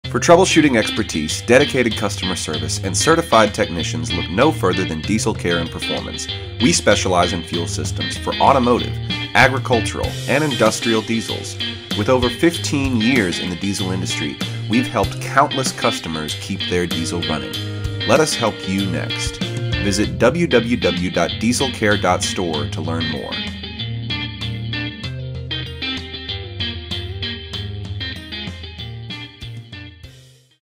Welcome to Diesel Care & Performance
We are a premier diesel remanufacturing company and parts supplier located in Memphis, TN. We provide rebuild services as well as parts for Chevy, Dodge, Ford, and Agricultural and Industrial diesels. We also offer technical support for customers who have questions about their diesel.

Our office hours are Monday through Thursday, 6:00AM-5:00PM, and 8:00AM-5:00PM Friday CST